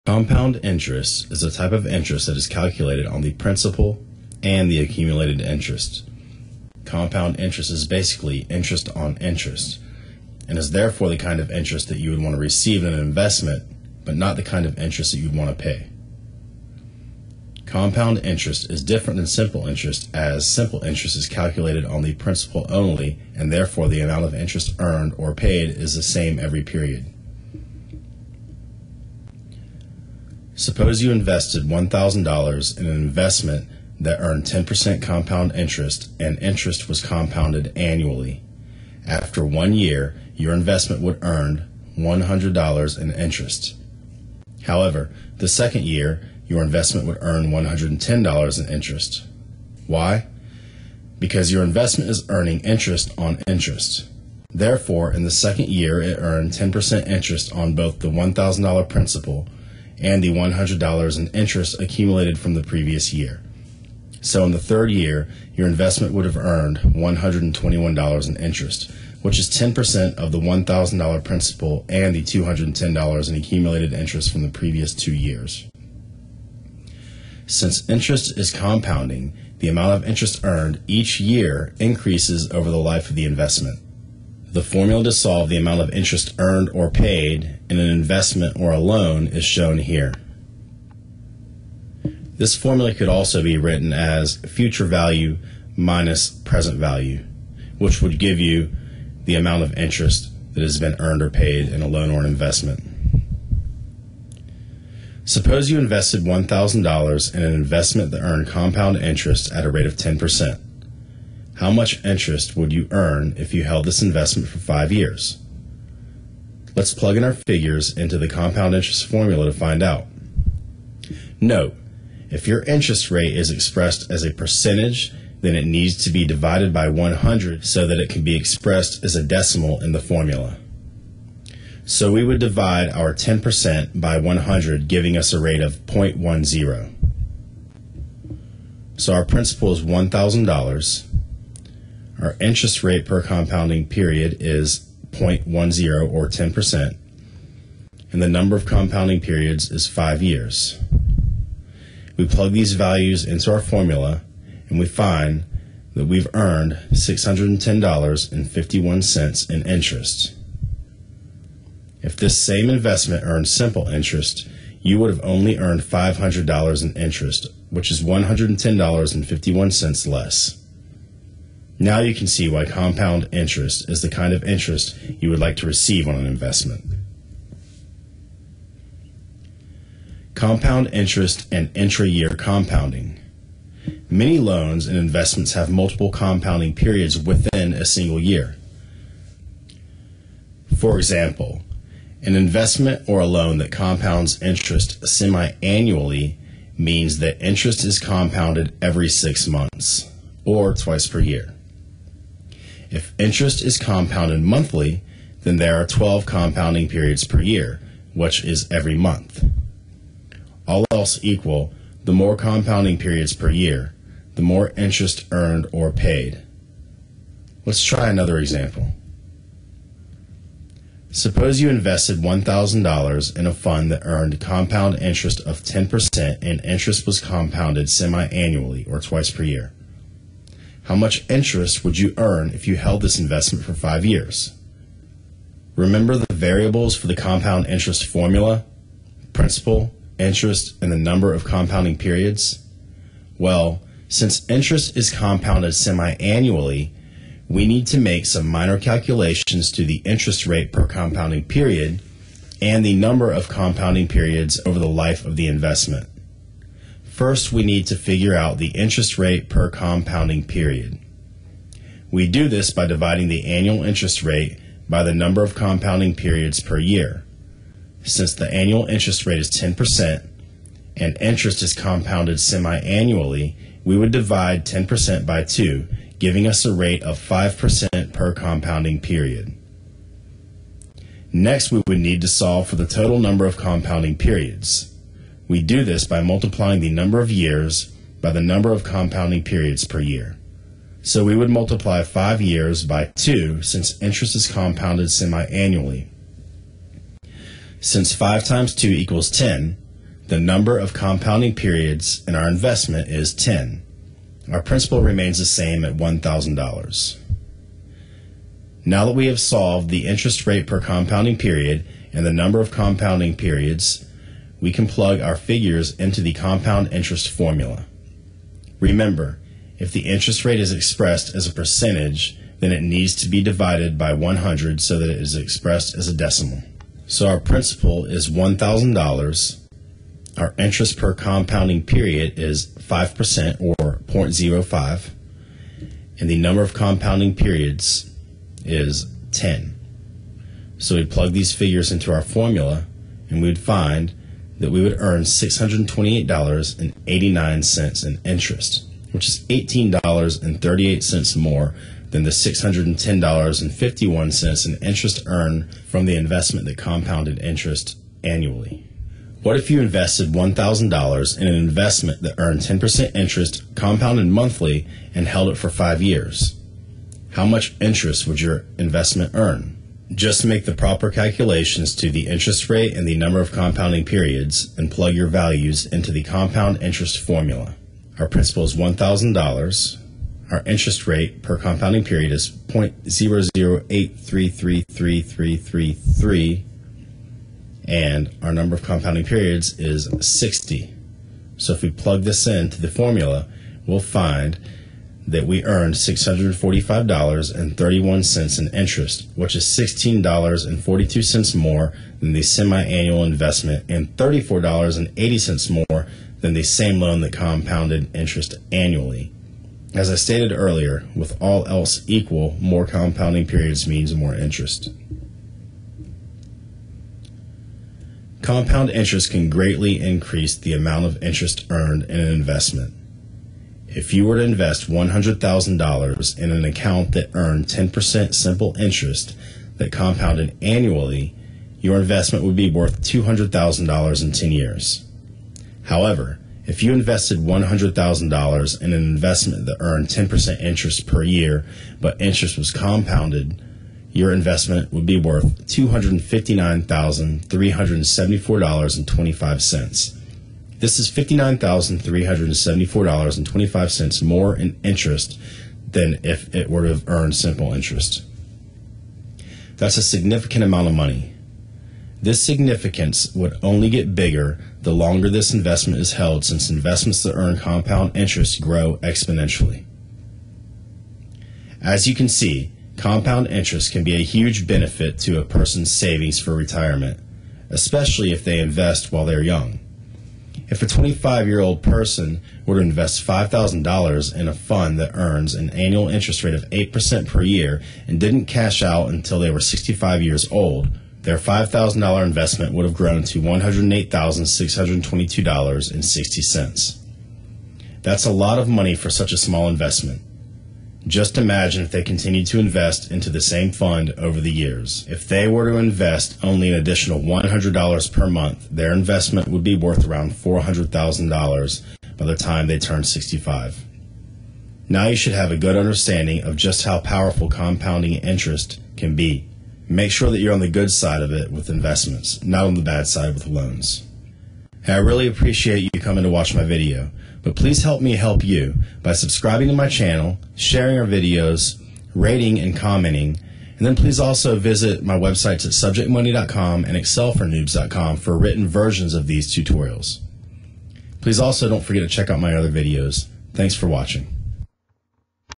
Compound Interest Lesson/Tutorial: What is the Compound Interest Formula?
In this tutorial I teach you about compound interest. I explain compound interest and the difference with compound interest and simple interest. I also show the compound interest formula for  how to solve for how much interest an investment or loan accumulates. I then cover intrayear compounding such as semi-annual and monthly compounding periods.